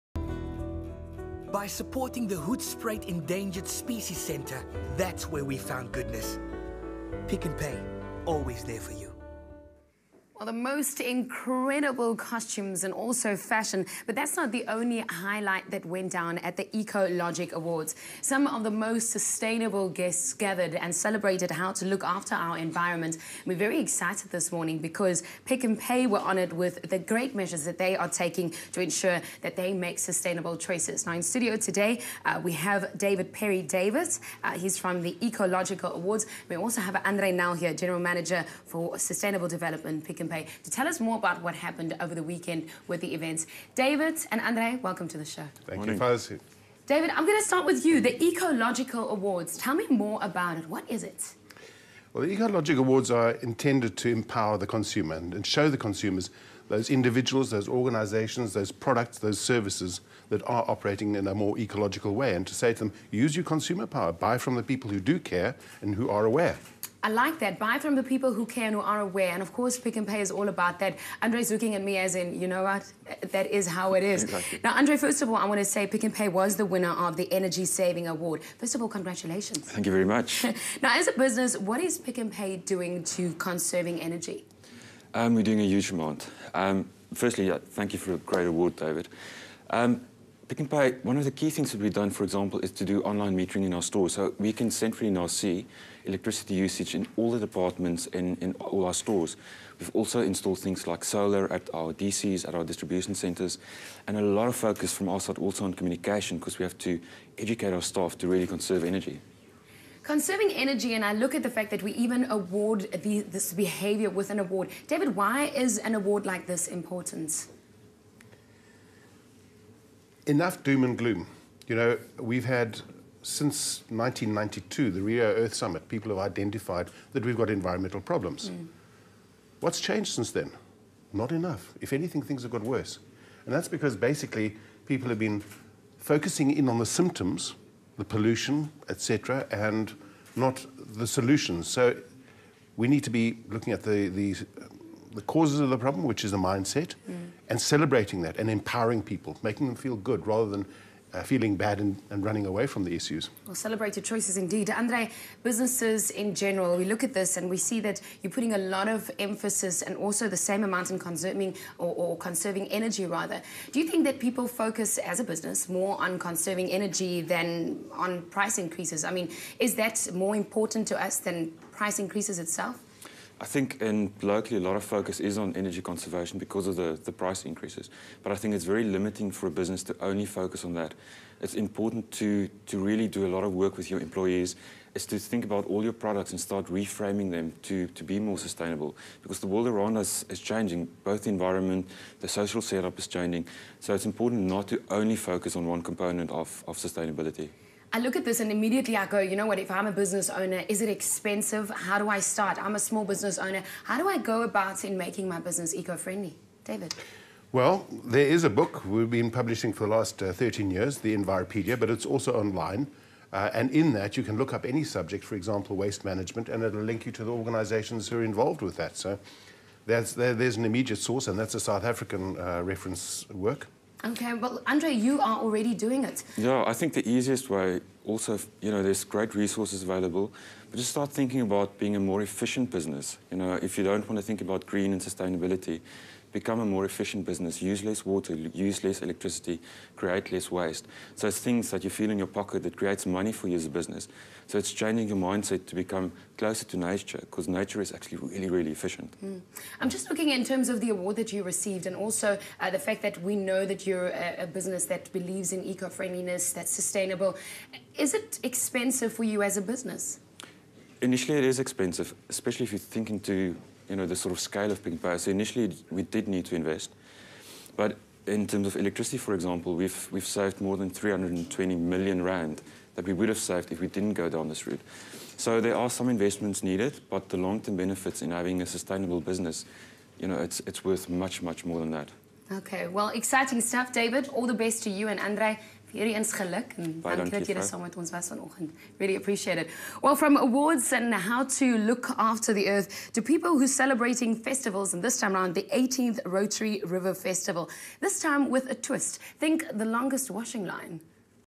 Pick n Pay Eco Logic Awards 30 09 13 5 Min
Pick n Pay is a sponsor of South Africa's prestigious Eco-Logic Awards. Catch them on SABC3